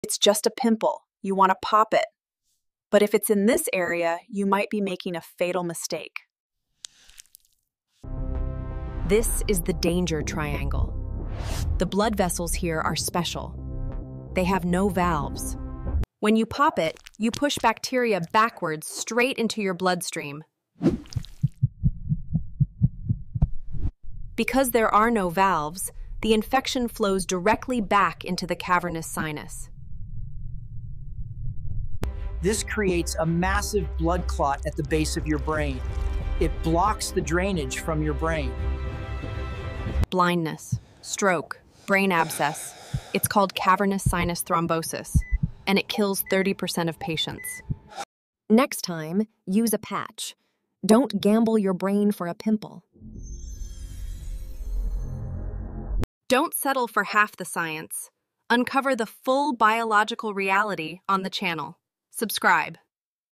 NEVER Pop a Pimple in this "Danger Triangle" ⚠️🔺
You want to pop that zit on your upper lip. Stop.

That area is called the "Danger Triangle" of the face. The veins here have no valves, creating a direct superhighway for bacteria to travel straight to your brain.

In this micro-simulation, we track the infection from a popped pimple to a deadly clot called Cavernous Sinus Thrombosis.